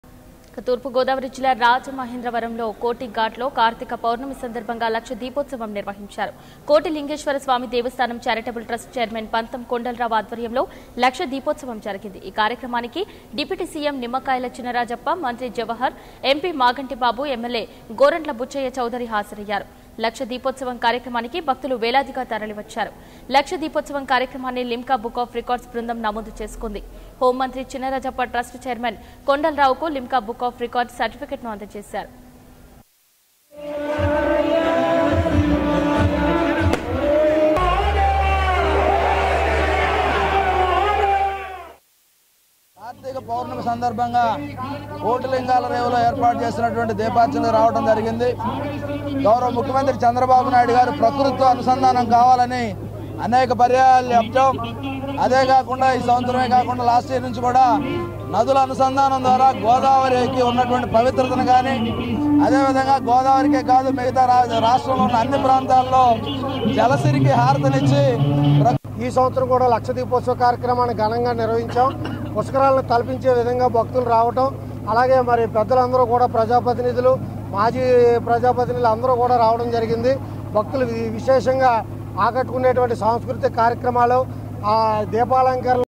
"లక్ష దీపోత్సవం" | lakh Lights Festival in East Godavari District ||#99 TV
Here Is the video of lakh Lights Festival in East Godavari District.

99%'s news is biased - bias not towards a political group but biased in favour of people.